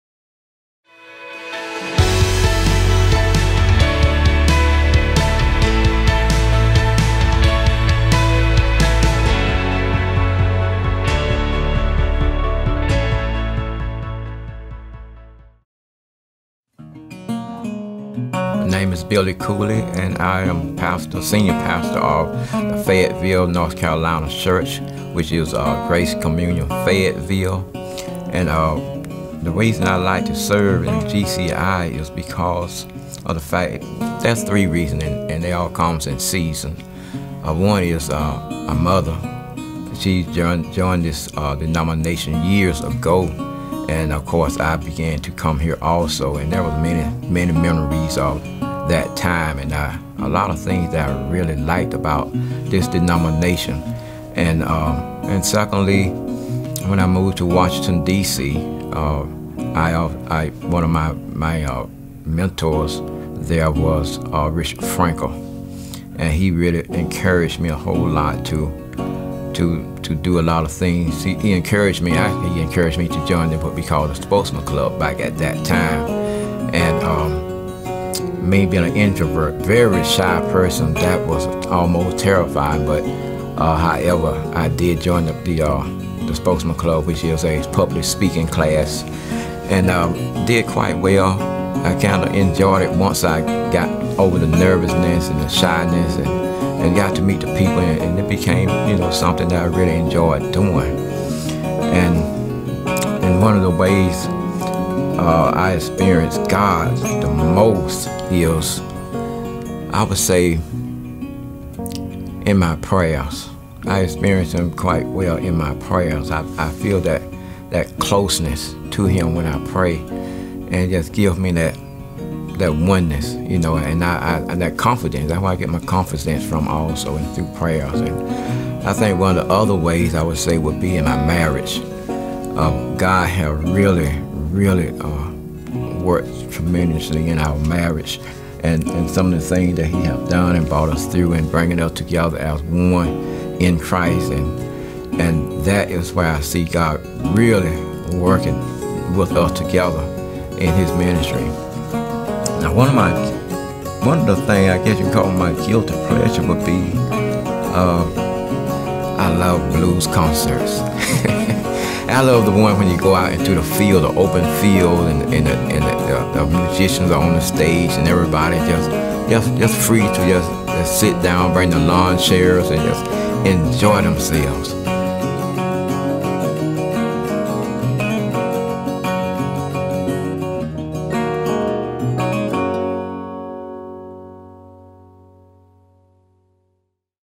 We Are GCI Series | Pastor Profile | Billy Cooley
We Are GCI Series is a collection of videos where various GCI leaders and members are highlighted.
In this episode, GCI Pastor, Billy Cooley, shares a little about himself and why he likes to serve in GCI and in what ways he connects the most with God.